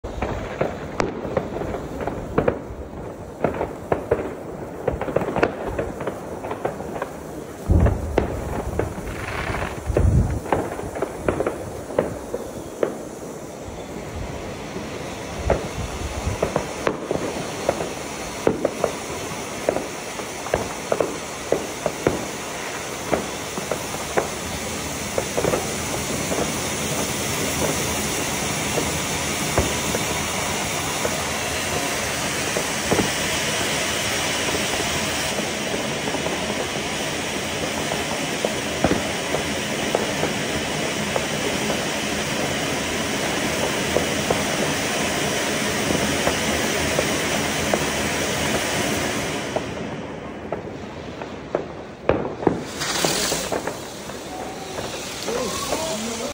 Straßenfeuerwerk Silvester 2022/23 Zink Nr. 7 Vulkan
Dieses Jahr haben wir unser Straßenfeuerwerk mit 3 F2 Vulkanen begonnen, an dritter stelle sieht man einen 250g  Vulkan von Zink,  die nummer 7 Schweizer Supervulkan Gold. Eingekauft haben wir unser Feuerwerk dieses Jahr bei Talfeuerwerk in Witten, ohne vorbestellung war das ein wirklich tolles Erlebniss/Abenteuer. Auch dieser Vulkan war einfach nur klasse, Mein Fazit... auch hier haben sich die 12,95€ für den Vulkan haben sich definitiv gelohnt.